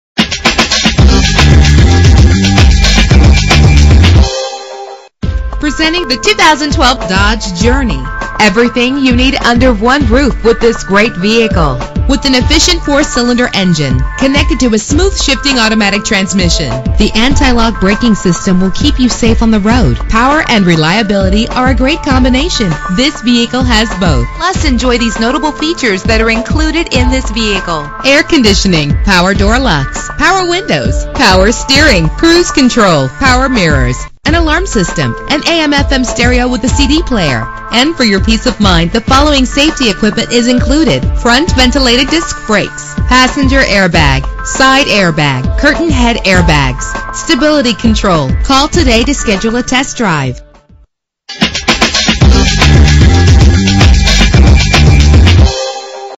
2012 Dodge Journey - Downersgrove IL
Year: 2012
Make: Dodge
Model: Journey
Trim: SE
Engine: 2.4 liter inline 4 cylinder 16 valve
Transmission: 4-Speed Automatic
Color: Black
Mileage: 7
Address:
2311 West Ogden